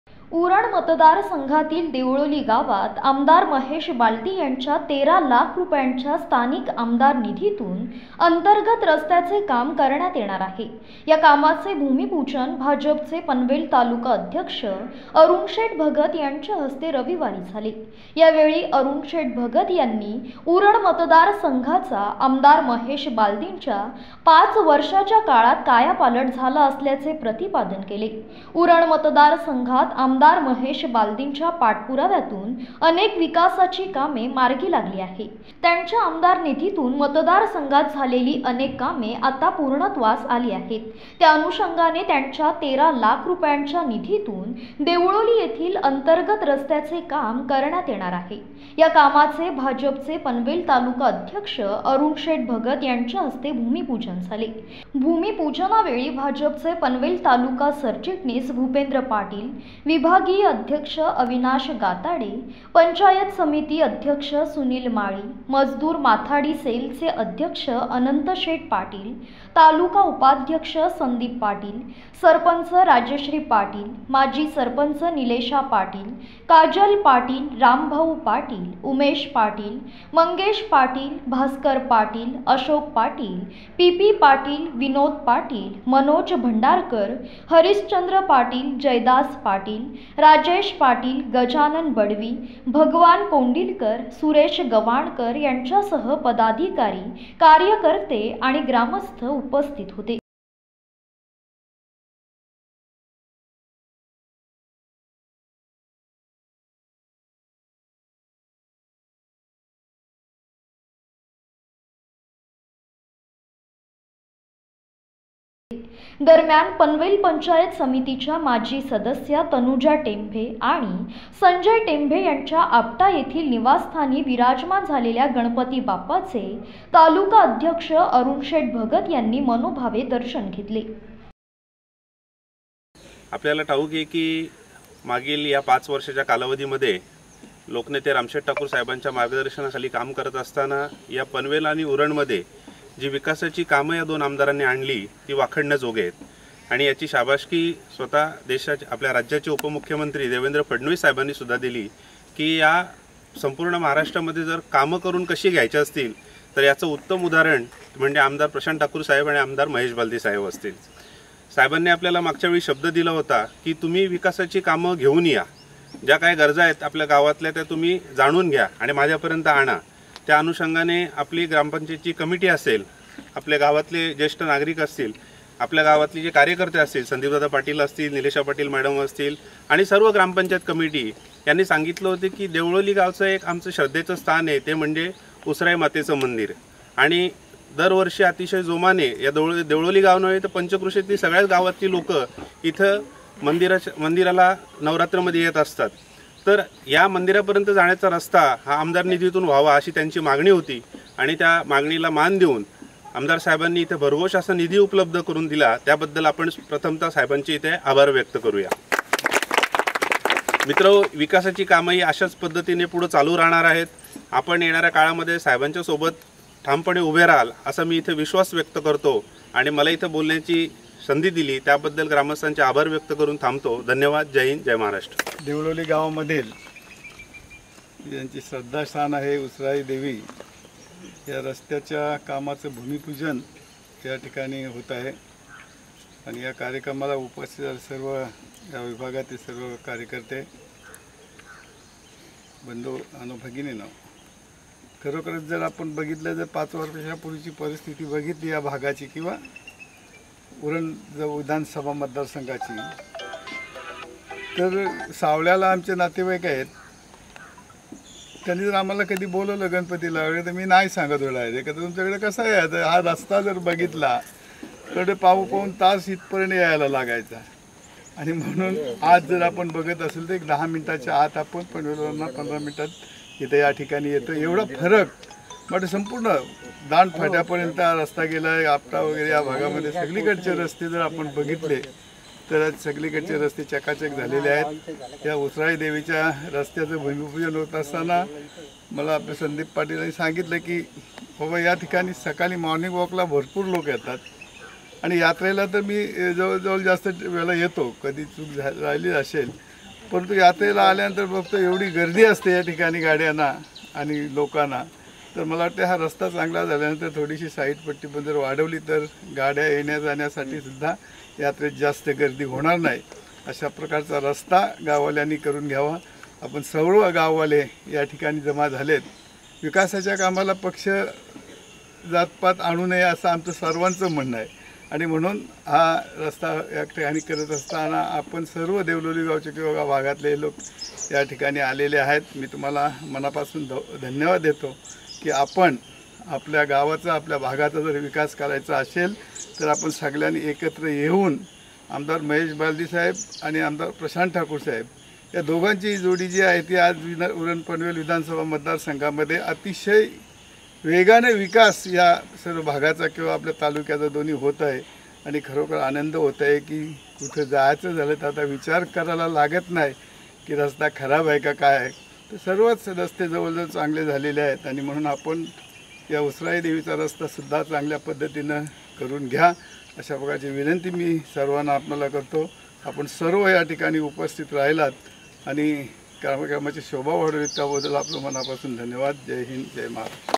भाजप नेते अरुणशेठ भगत यांच्या हस्ते देवळोली येथे रस्ता कामाचे भूमिपूजन !
उरण विधानसभा मतदारसंघातील देवळोली गावात आमदार महेश बालदी यांच्या 13 लाख रुपयांच्या निधीतून अंतर्गत रस्त्याचे काम करण्यात येणार आहे. या कामाचे भूमिपूजन भाजपचे पनवेल तालुका अध्यक्ष अरुणशेठ भगत यांच्या हस्ते झाले. या वेळी त्यांनी उरण मतदारसंघाचा आमदार महेश बालदी यांच्या पाच वर्षाच्या काळात कायापालट झाला असल्याचे प्रतिपादन केले.